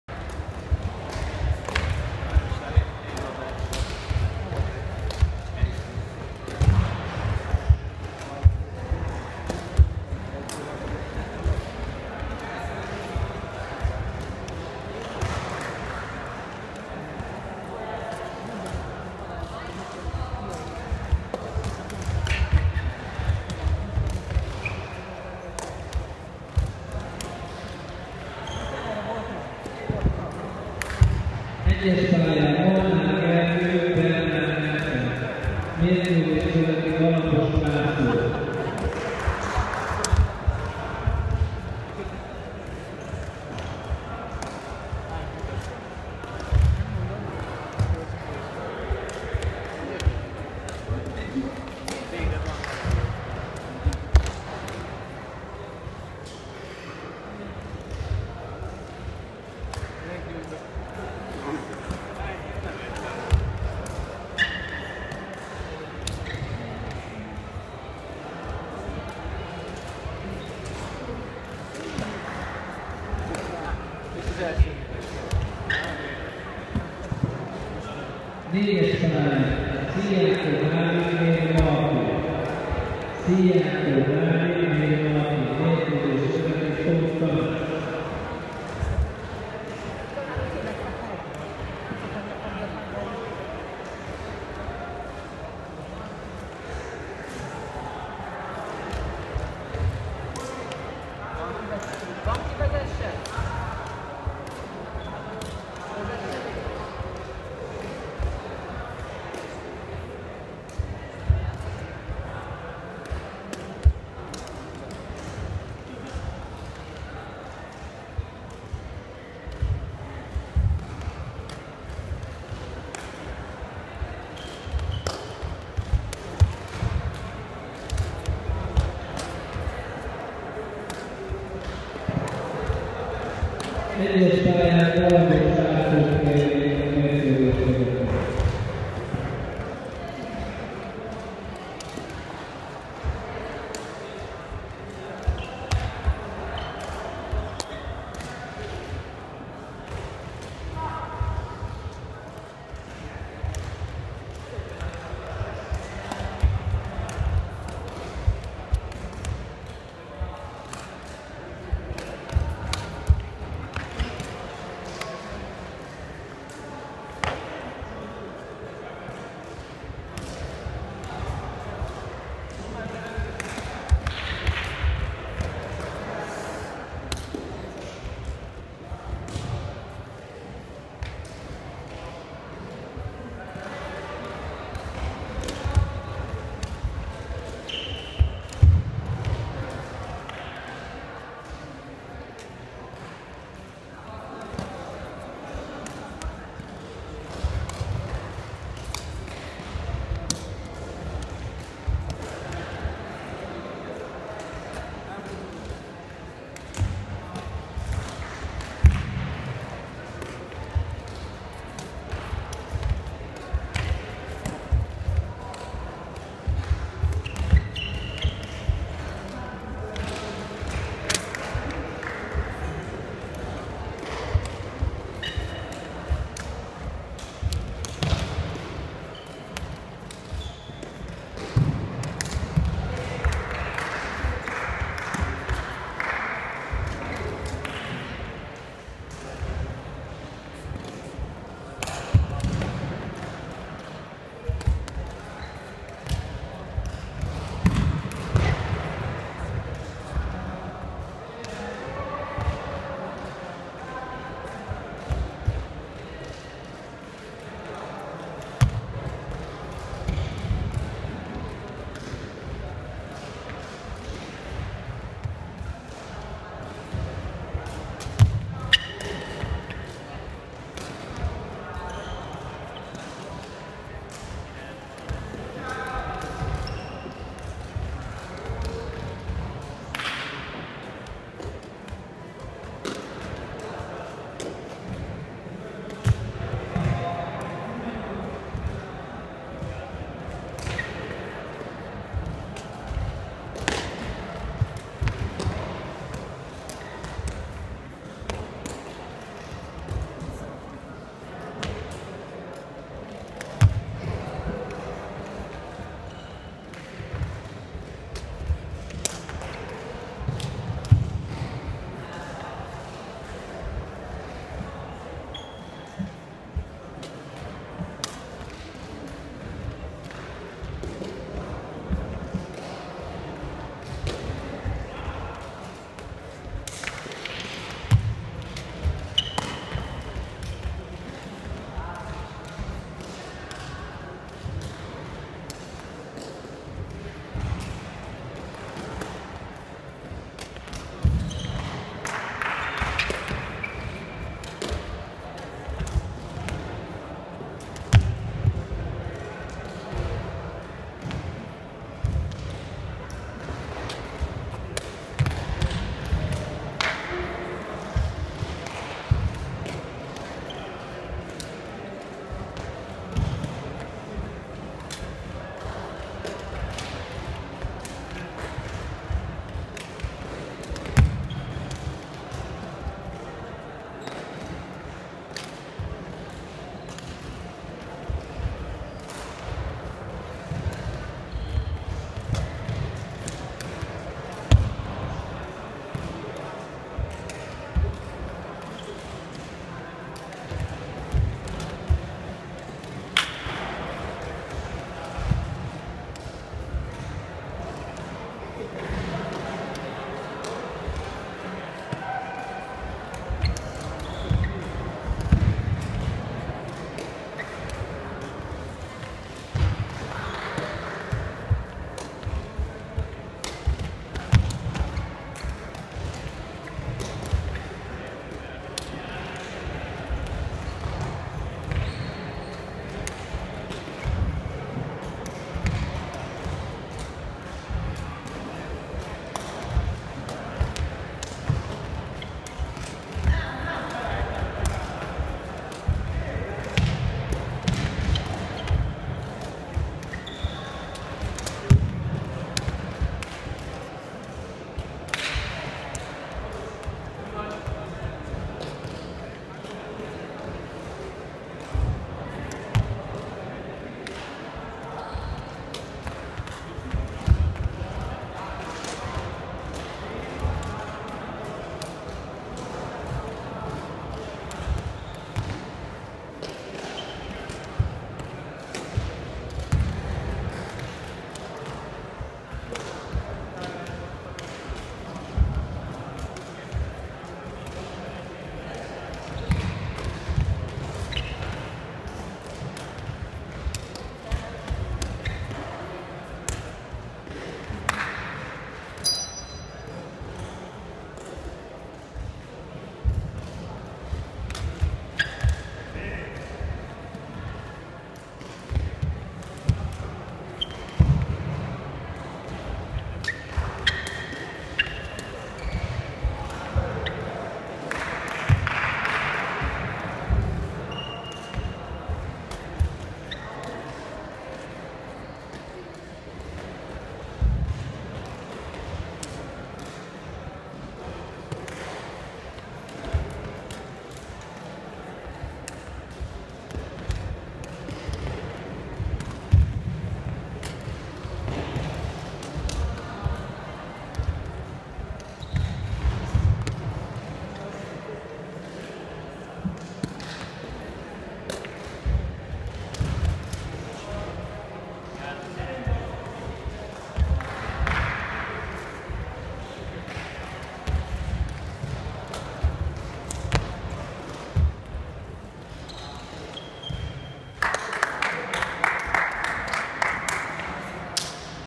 Shuttlecock Country Olympiad 2019, 1/8: Bakos Dávid - Molnár Dániel
Lábtoll-labda Országos Diákolimpia, Kistelek

U17 Nyolcaddöntő:
Bakos Dávid (Újszász Rózsa)
Molnár Dániel (Szolnok Varga)